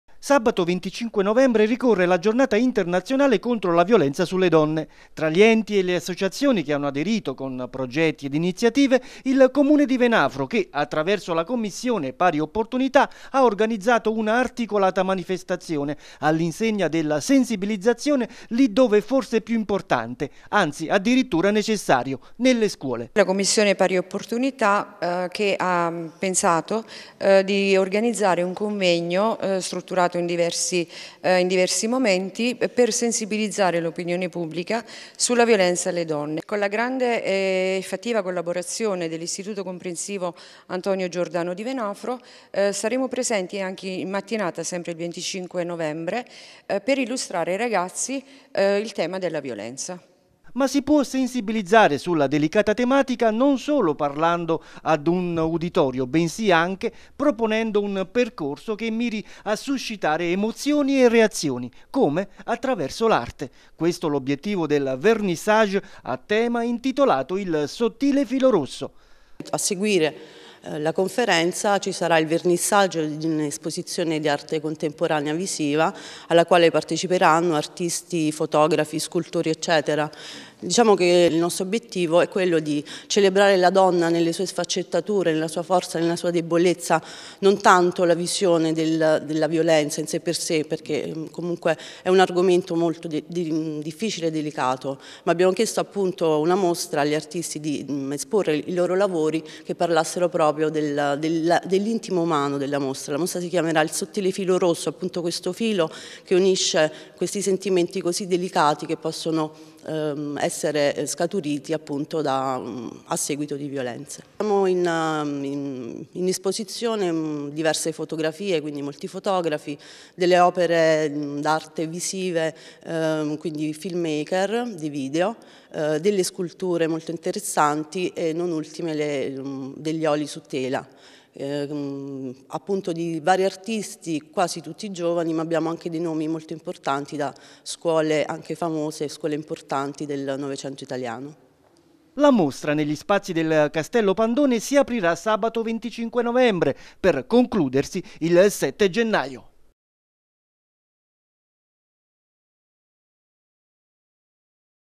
Contro la violenza di genere, un'iniziativa a Venafro
A Venafro presentata un’ iniziativa fortemente voluta dalla locale Commissione Pari Opportunità  contro la violenza sulle donne ed all’insegna della sensibilizzazione..anche attraverso l’arte. Si terrà sabato 25 novembre…